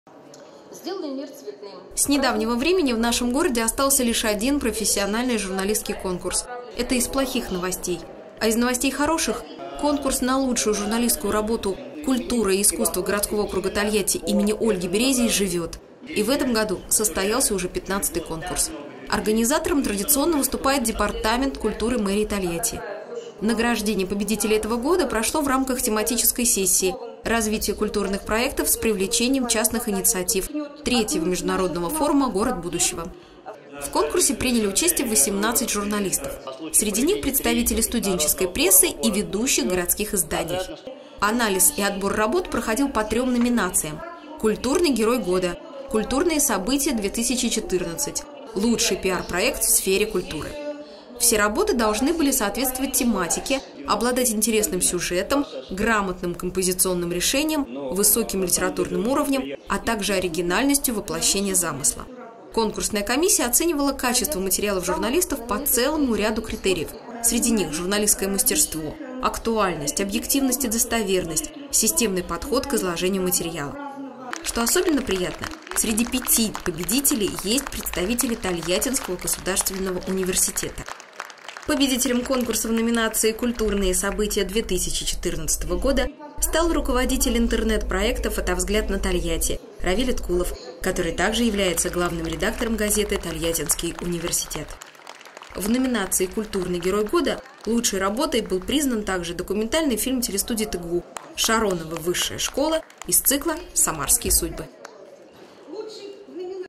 ТГУ NEWS: Журналисты ТГУ на XV конкурсе имени Ольги Березий
5 декабря 2014 года были подведены итоги конкурса на лучшую журналистскую работу “Культура и искусство городского округа Тольятти” имени Ольги Березий.
Победителем конкурса в номинации «Культурные события 2014 года» стал руководитель интернет-проекта «Фотовзгляд на Тольятти» Равиль Иткулов, который также является главным редактором газеты «Тольяттинский университет». В номинации «Культурный герой года» лучшей работой был признан также документальный фильм телестудии  ТГУ «Шароновы. Высшая школа» из цикла «Самарские судьбы».